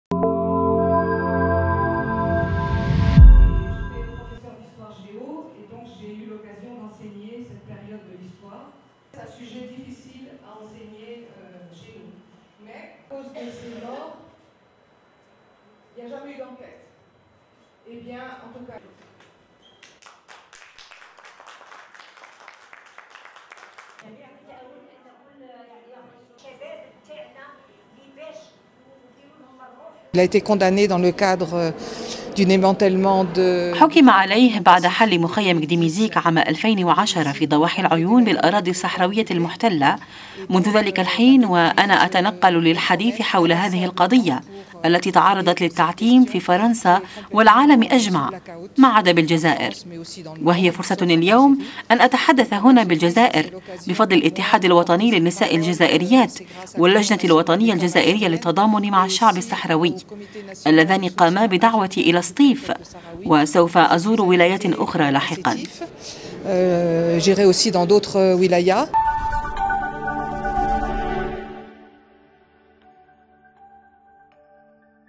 سطيف: تكريم زوجة المعتقل السياسي "النعمة عبدي الاصفاري" نظير نضالها حول القضية الصحراوية
سطيف (وأج)- أسدى الإتحاد الوطني للنساء الجزائريات، درع استحقاق للناشطة الحقوقية كلود مونجان، زوجة المعتقل الصحراوي "النعمة عبدي الاصفاري" القابع بسجون المخزن المغربي، وذلك نظير نضالها حول القضية الصحراوية.